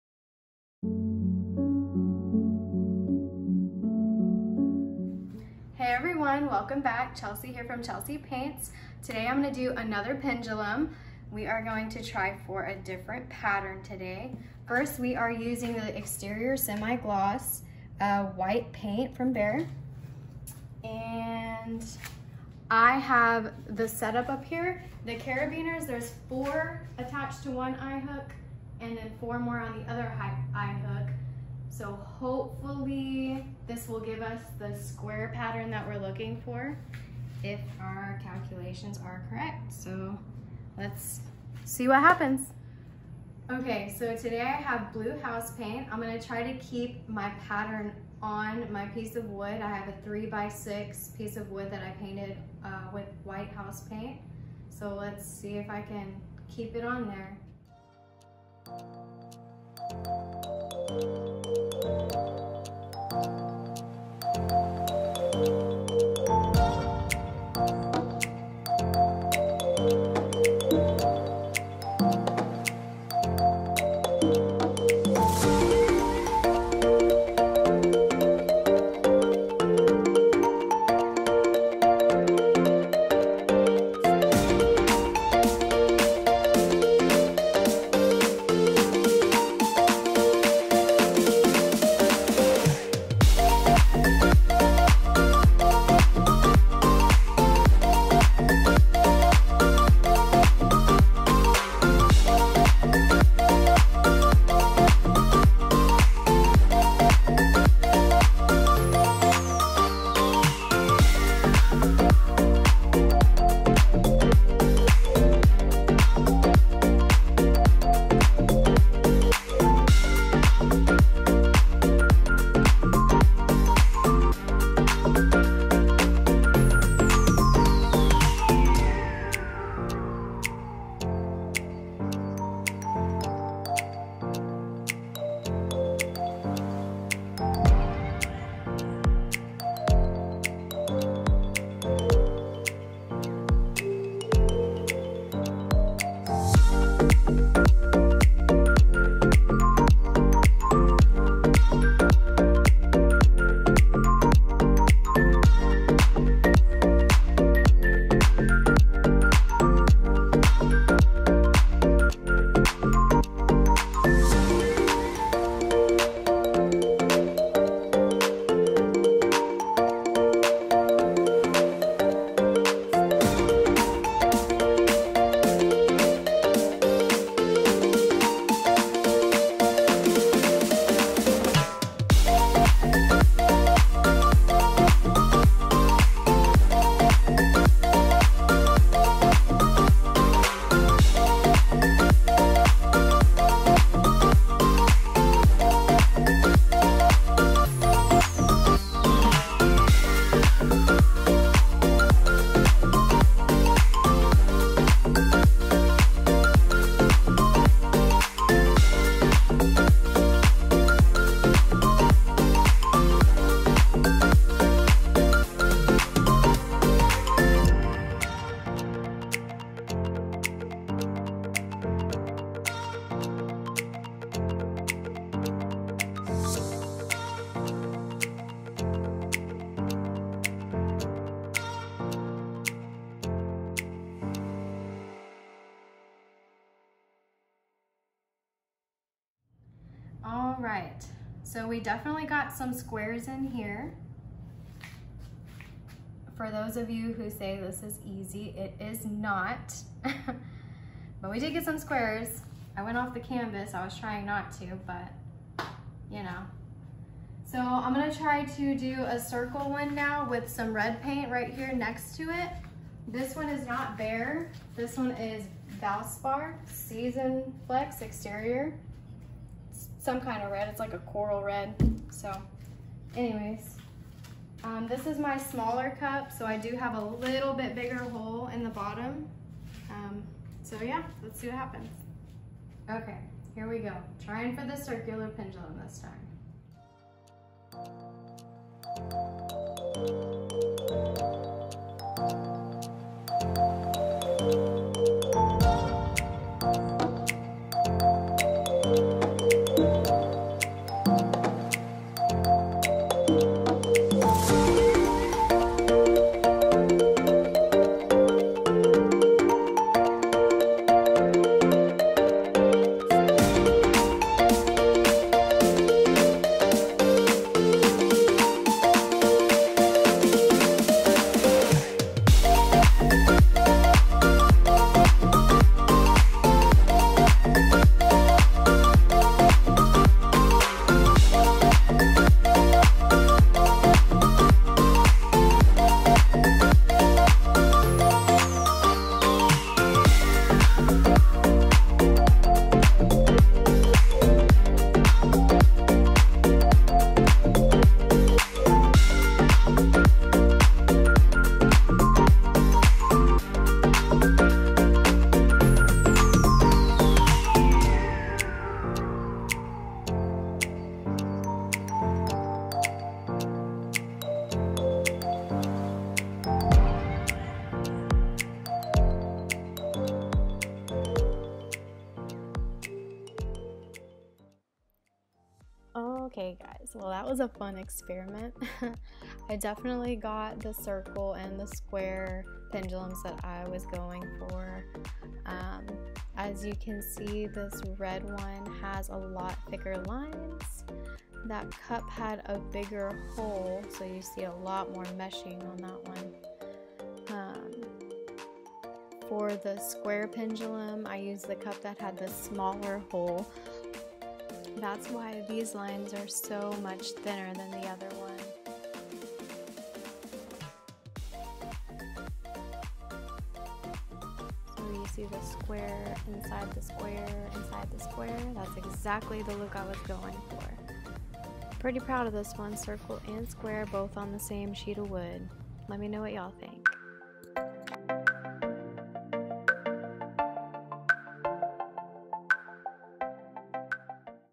PENDULUM PAINTING on MASSIVE SHEET of PLYWOOD / CIRCLE & SQUARE / CHELC PAINTS / Mesmerizing Artist
PENDULUM PAINTING CIRCULAR & SQUARE PATTERN. I wanted to try to do a round pattern and a square pattern on the same. I used just house paint for this pendulum. White paint by Behr from Home Depot for the main color and then blue Valspar from Lowes for the square. The circular pendulum is red paint from Valspar as well from Lowes. If you have any questions about the way I have the pendulum set up I have a video going over the complete set up on my channel!!!

To see how I set up my Pendulum Paintings in my Garage please check out the video link below and let me know what you think!!!

Support me and my painting journey 🌈🎨😁